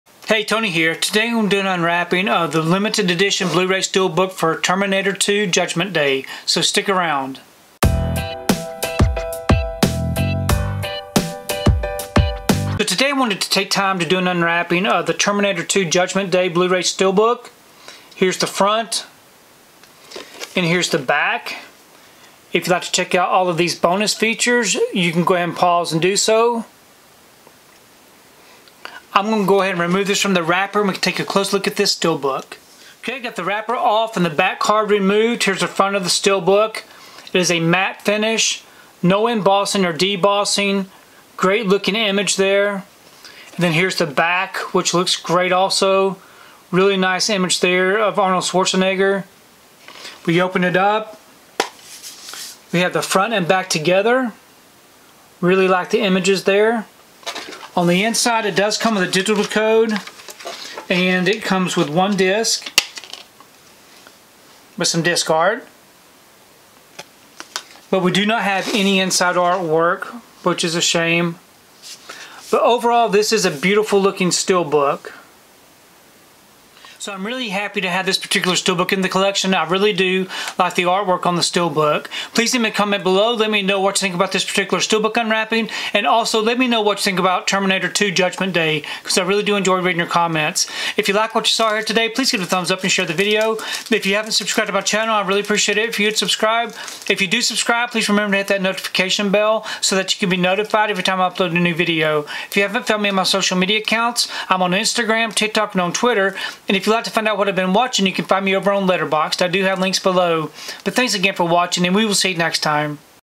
Terminator 2: Judgment Day Best Buy Exclusive Limited Edition Blu-ray Steelbook from 2013
Check out my unwrapping of the Terminator 2: Judgment Day Best Buy Exclusive Limited Edition Blu-ray Steelbook!

► Purchase the 4K Ultra HD + Blu-ray
► Purchase the Blu-ray